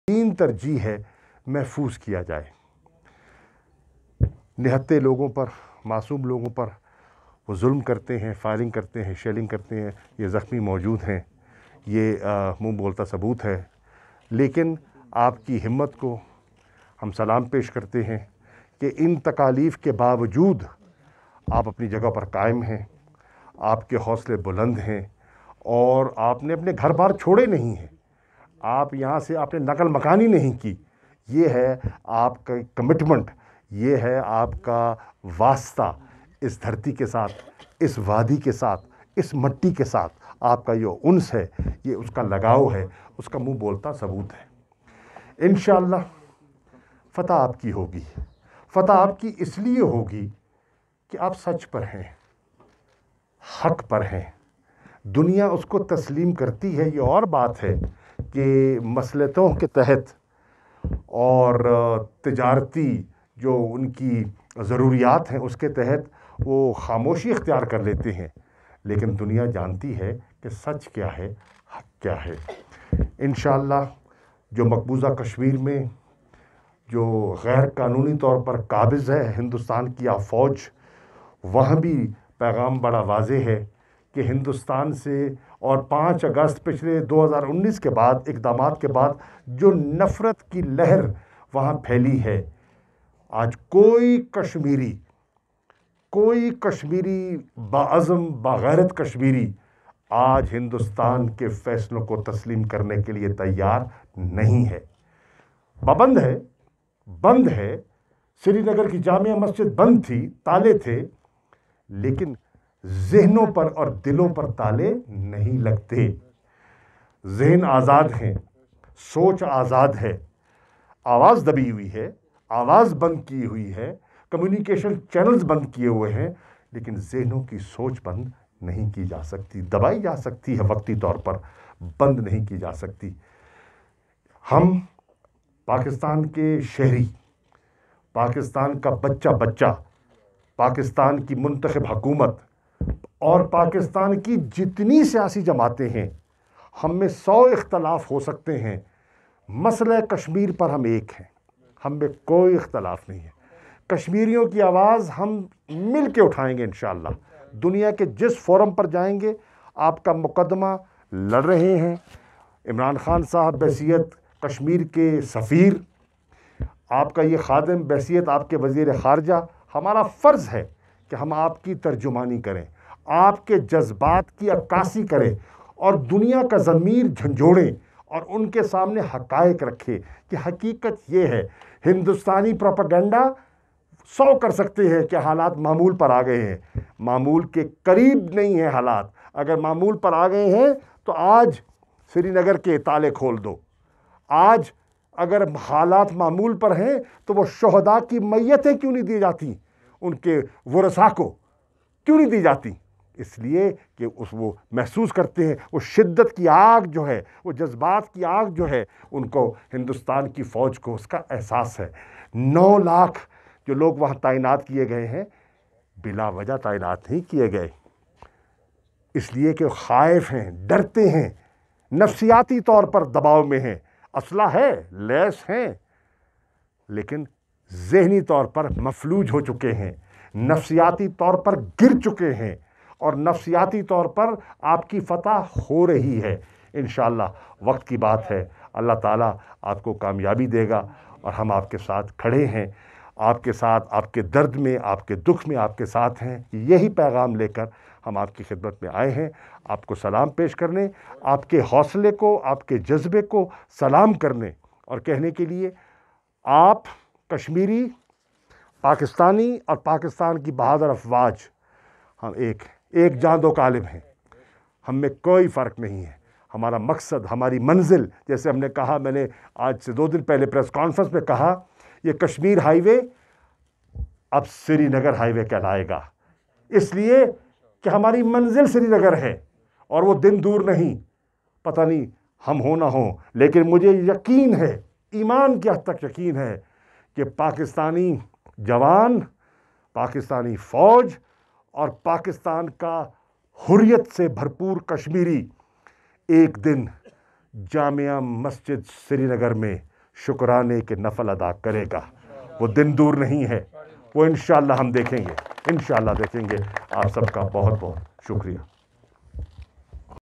FM Qureshi calls on PM AJK after LoC visit
MUZAFFARABAD  Foreign Minister Shah Mehmood Qureshi, Defense Minister Pervaiz Khattak and Special Assistant to Prime Minister (SAPM) on National Security Moed Yusuf visited Line of Control (LoC) at Chirikot sector on Monday to express solidarity with Kashmiris.

Military officials briefed Shah Mehmood Qureshi and Pervaiz Khattak in detail. The foreign minister paid tribute to soldiers of Pakistan Army and said that entire nation is proud their bravery.